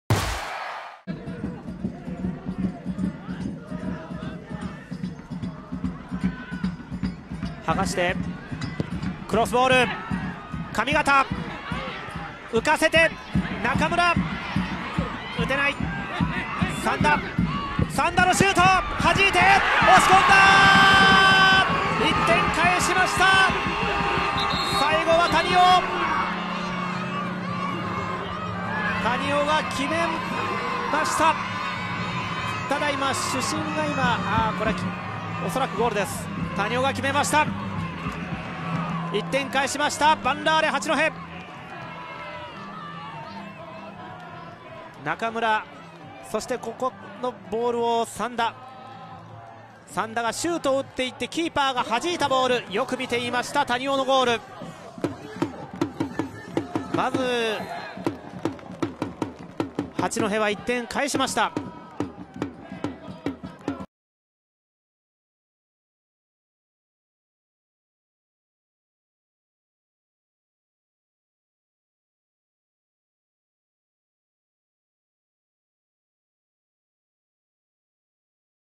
【公式】ゴール動画：谷尾 昂也（八戸）60分 ヴァンラーレ八戸vsＹ．Ｓ．Ｃ．Ｃ．横浜 明治安田生命Ｊ３リーグ 第3節 2019/3/24
八戸が左サイドでの攻勢からシュートチャンスを作り出すと、相手GKが弾いたボールを谷尾 昂也（八戸）が押し込んで1点を返す！明治安田Ｊ３のゴール動画を公開中！
2019年3月24日（日）に行われた明治安田生命Ｊ３リーグ 第3節 八戸vsYS横浜のゴール動画です。